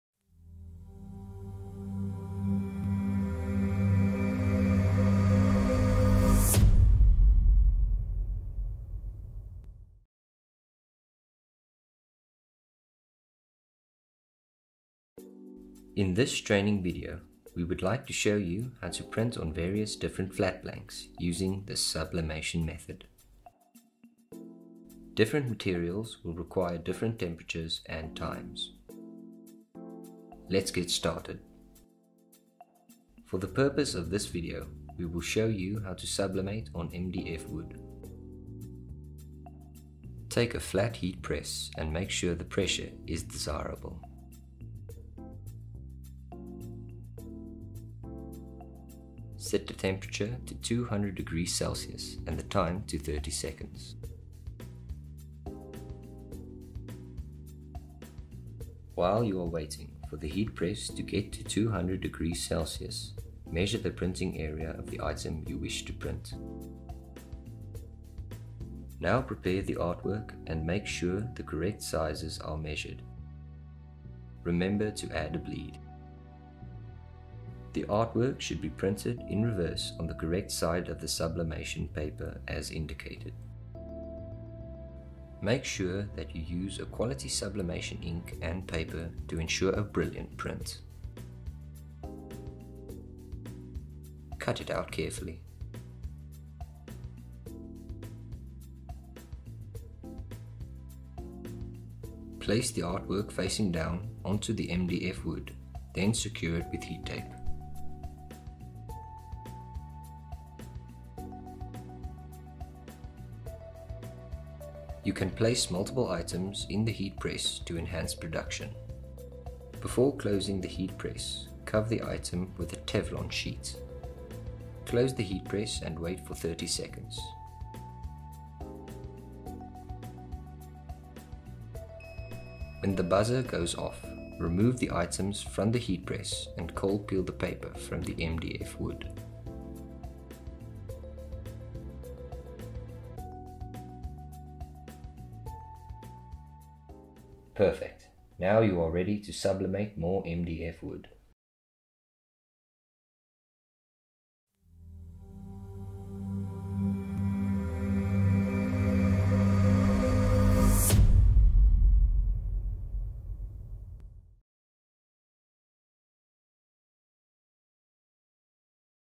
Flat Press Training Titan-Jet Africa
In this training video we show you how to sublimate flat objects. Prepare the artwork and scale it to the correct size. Print the artwork on sublimation paper. We will also show you how to apply the print to a keyring, puzzle or coaster.

You can find all the items and machines on the online store.

Items / Machines:
- Flat press
- Sublimation flat blanks
- Sublimation paper
- Heat tape
- Epson Printer